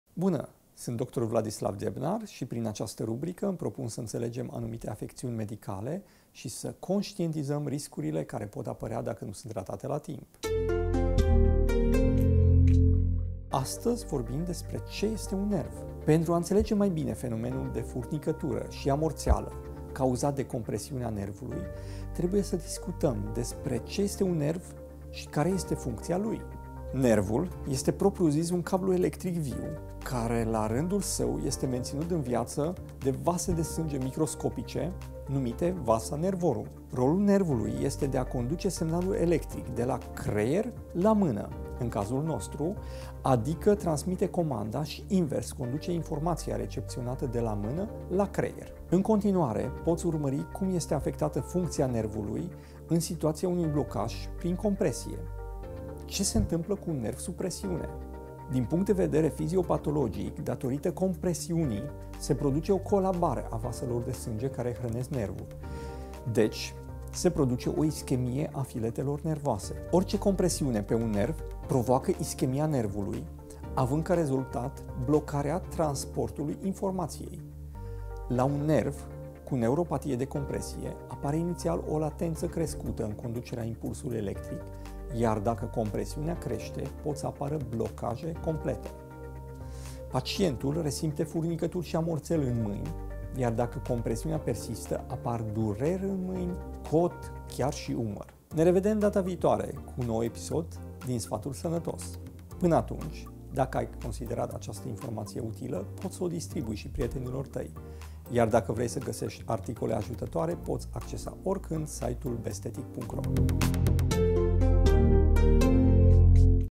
Furnicaturi 👋 part 4 👉 Ce este un nerv? #2.4 Sfatul sanatos
Ce este un nerv?
Pentru a intelege mai bine fenomenul de furnicatura si amorteala cauzat de compresiunea nervului, trebuie sa discutam despre ce este un nerv si care e functia lui.
Nervul este  propriu zis un cablu electric viu care la randul sau este mentinut in viata de vase de sange microscopice - numite vasa nervorum.
Rolul nervului este de a conduce semnalul electric de la creier la mana, in cazul nostru, adica transmite comanda, si invers conduce informatia receptionata de mana la creier.
In  continuare, poti urmari cum este  afectata functia nervului in situatia unui blocaj prin compresie.
Ce se intampla cu un nerv sub presiune?
D.p.d.v. fiziopatologic,datorita compresiunii se produce o colabare a vaselor de sange care hranesc nervul, deci se produce o ischemie a filetelor nervoase.
Orice compresiune pe un nerv provoaca ischemia nervului, avand ca rezultat blocarea transportului informatiei.
La un nerv cu neuropatie de compresie, apare initial o latenta crescuta in conducerea impulsului electric, iar daca compresiunea creste, pot sa apara blocaje complete.
Pacientul resimte furnicaturi si amorteli in maini iar daca compresiunea persista, apar dureri in maini, cot si chiar in umar.